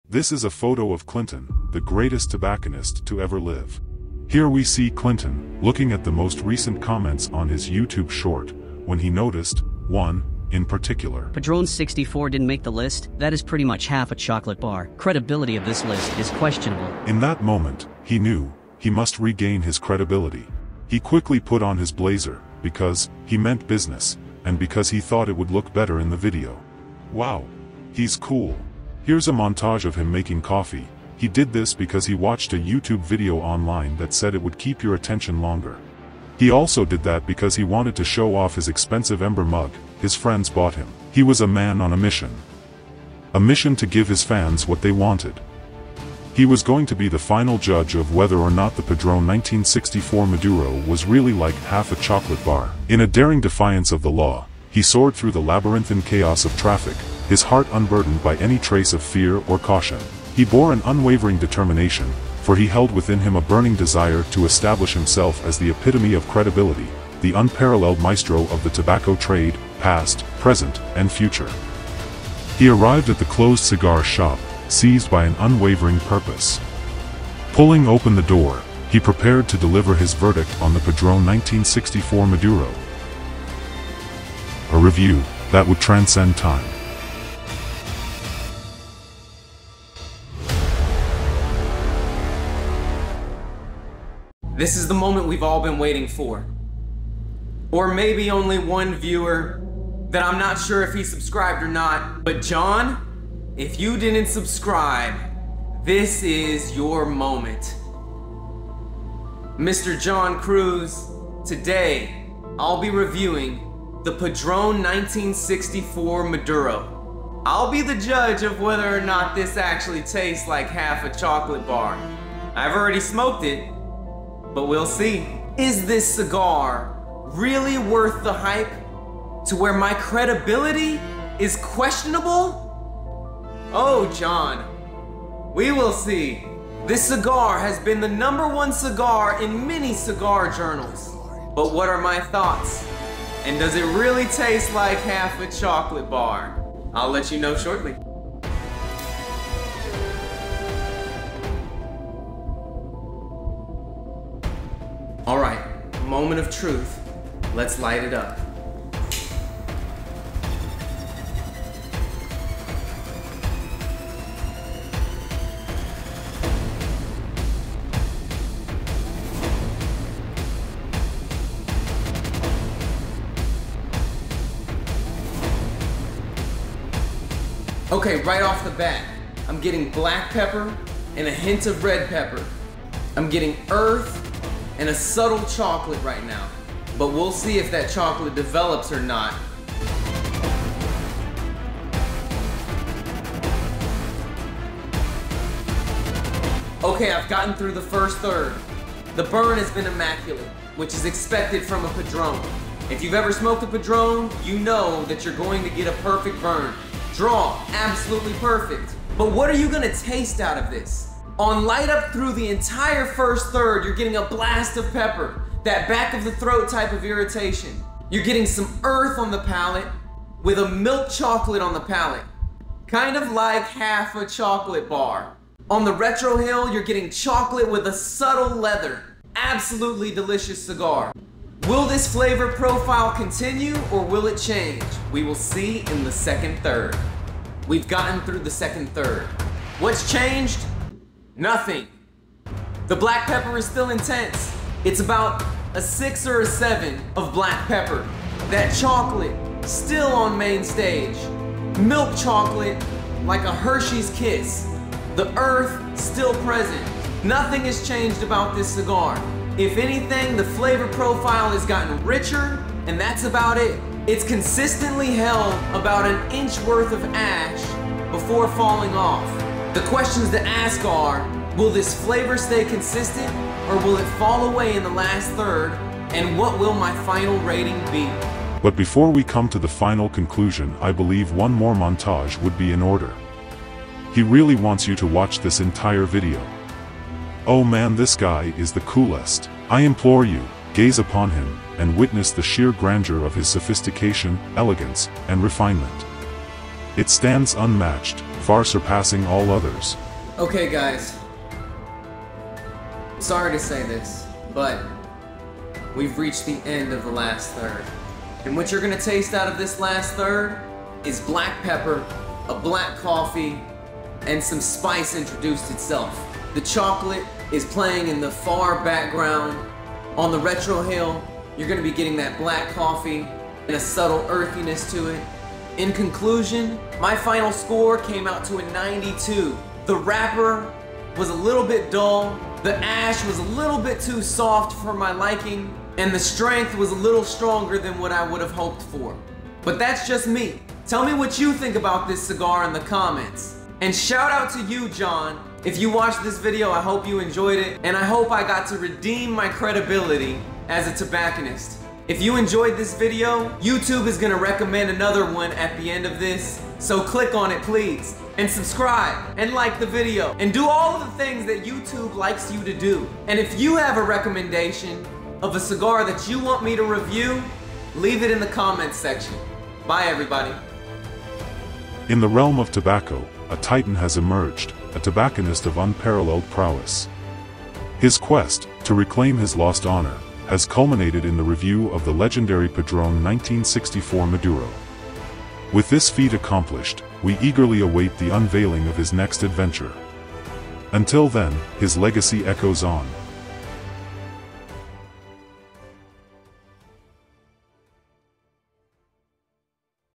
I was CRITICIZED for THIIS! - Padron 1964 Presidente Maduro Review
Join me on an entertaining journey as I embark on a quest to prove myself as the greatest tobacconist ever to exist. This video is my playful response to a critic who questioned my previous list of cigars that taste like chocolate, specifically pointing out the omission of the Padron 1964 Maduro. In a light-hearted and cinematic style, I decided to create an entire review dedicated to this exceptional cigar, adding a touch of humor along the way.